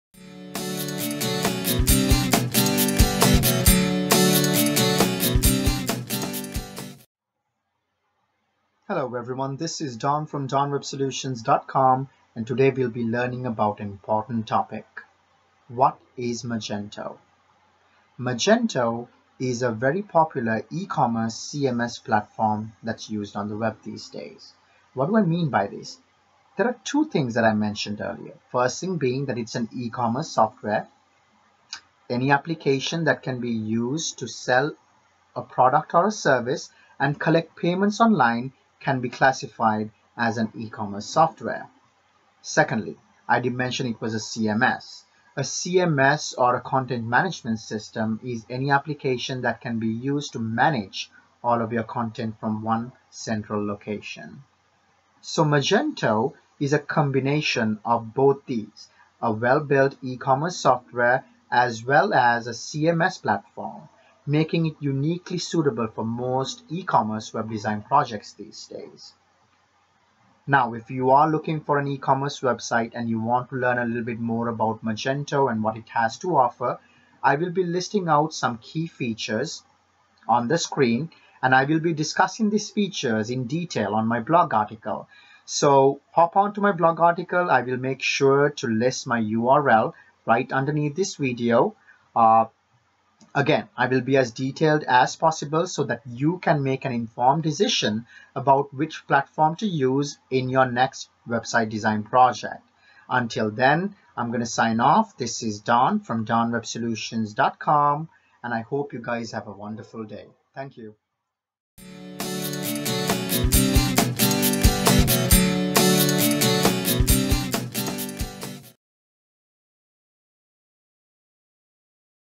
What is Magento?
A brief introduction to the Magento eCommerce Platform. Are you looking for a good eCommerce platform for your next website? Find out more about what Magento has to offer.

Want to build an E commerce website but not sure where to start?  Maybe you’ve heard a lot about Magento as a possible solution for your e-commerce website but, not exactly sure what to expect? Want to learn more about Magento? I hope to answer some of your questions in this article.

Magento is a well known eCommerce platform used by many as a robust solutions for their online eCommerce business. More than 240,000 merchants worldwide put their trust in our eCommerce software. Magento’s eCommerce platform gives you the tools you need to attract more prospects, sell more products, and make more money. Magento Community Edition is open source eCommerce software used to power hundreds of thousands of online stores. The software is frequently used by developers and small businesses to power their online web stores. The core code can be modified by developers and enhanced by installing application extensions from the Magento Connect marketplace.

What is an Ecommerce Software?
Any application that can be used to conduct commercial transactions electronically on the Internet or simply, any application that can be used to sell a product or service online.

There are 2 versions on the Magento CMS Platform.
Magento Community Edition – Open source Software free to download from the magento website. Mostly used by developers and small businesses to power their eCommerce Websites. At Don Web Soltuions, we provide design, development and support for the community platform.
Magento Enterprise Edition– Paid robust software suited for Big businesses that will offer more features and has expert support from Magento.